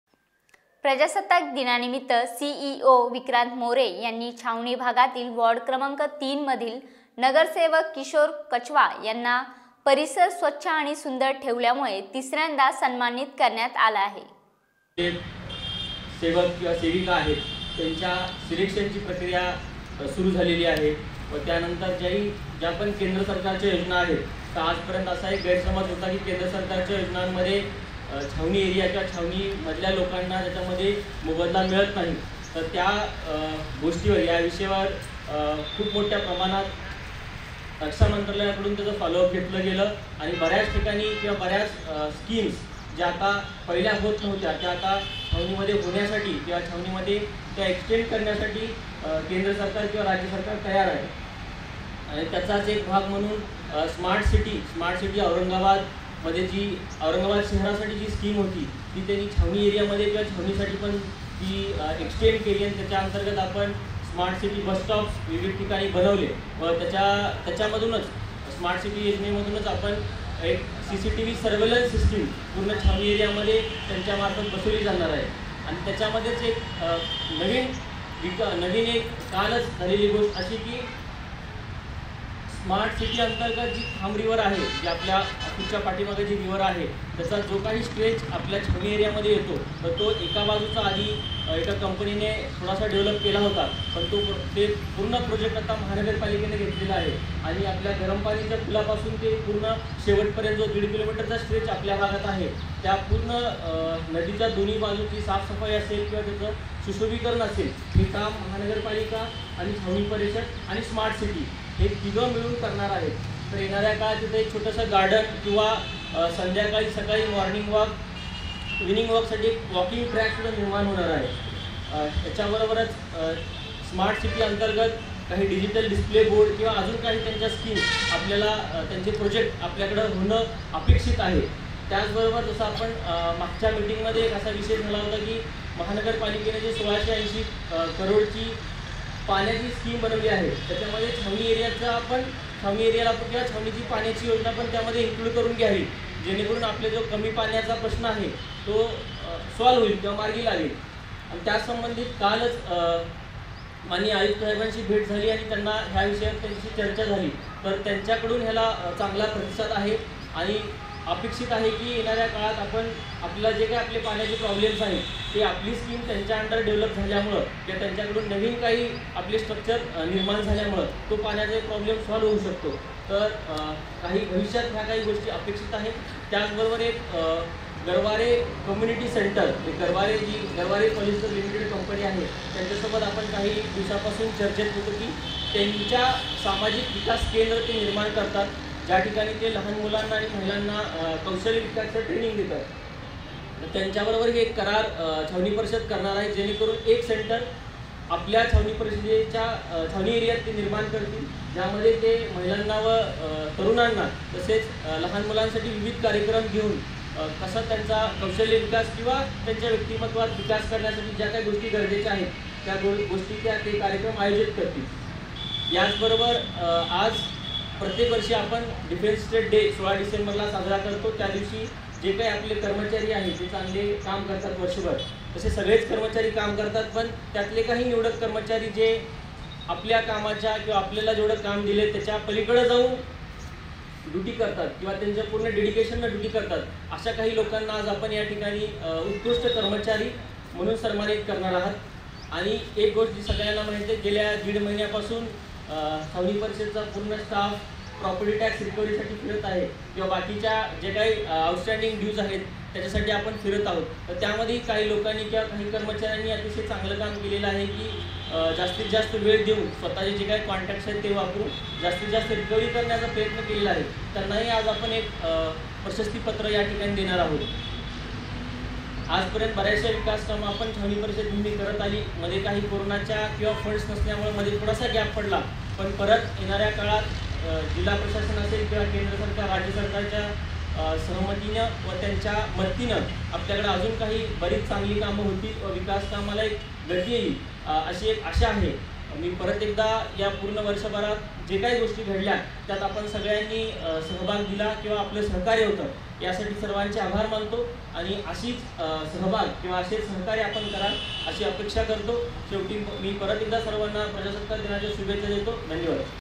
मराठा तेज - सीईओ विक्रांत मोरे यांच्या हस्ते नगरसेवक किशोर कच्छवा यांचा सन्मान.
MARATHA TEJ -  सीईओ विक्रांत मोरे यांच्या हस्ते नगरसेवक किशोर कच्छवा यांचा सन्मान.